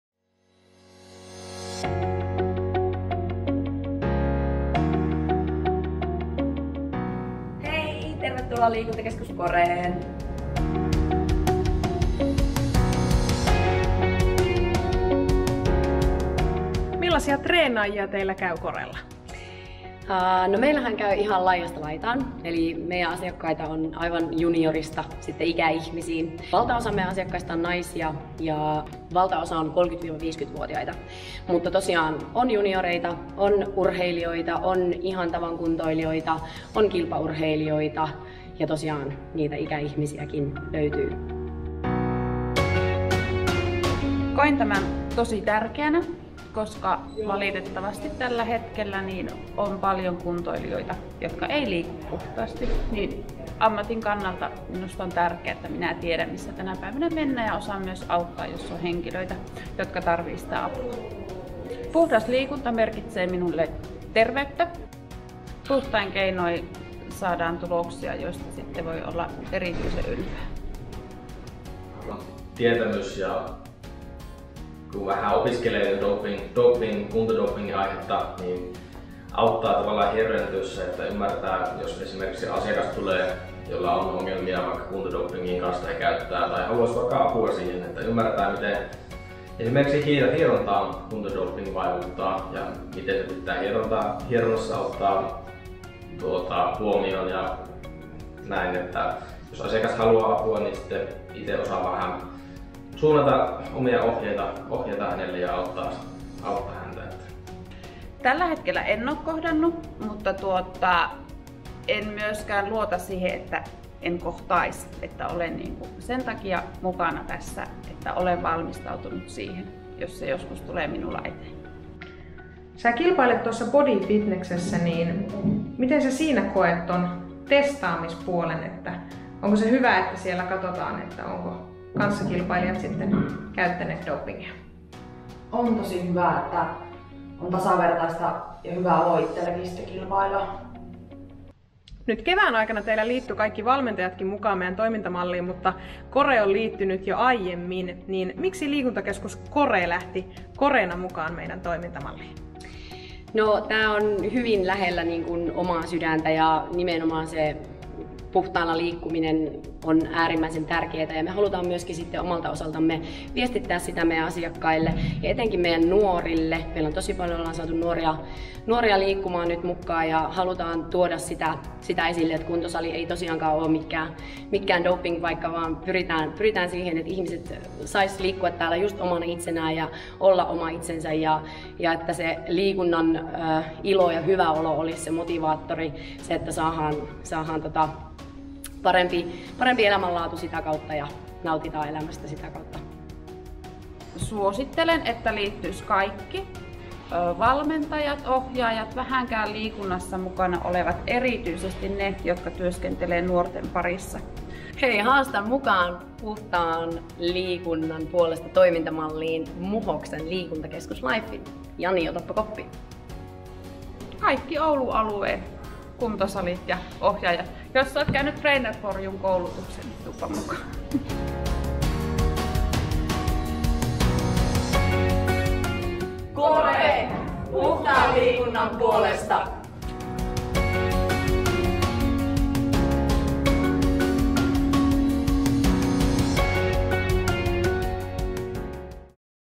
Liikuntakeskus Corella liikutaan puhtaasti
Liikuntakeskus Core sekä Coren valmentajat ovat liittyneet keväällä 2019 mukaan Puhtaan liikunnan puolesta-toimintamalliin ja videolta pääset kuulemaan valmentajien mietteitä toimintamalliin kuulumisen hyödyistä sekä puhtaasta liikunnasta!

Liikutaan yhdessä puhtaasti nyt ja tulevaisuudessa! 💪